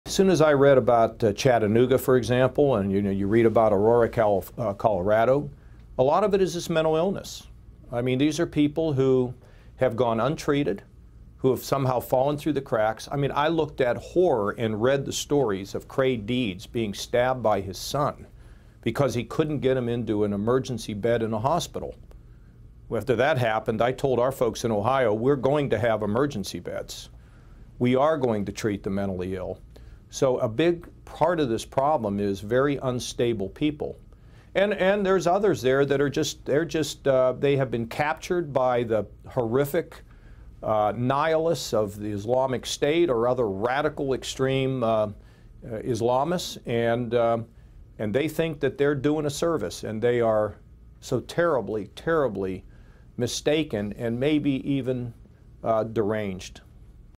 John Kasich on mass shootings: "A lot of it is mental illness"
Ohio Gov. and Republican presidential candidate John Kasich said a large contributing factor to mass shootings in the U.S. is mental illness, and he said he has directed his state to treat the mentally ill. Kasich also said the rise of extremism is partially to blame.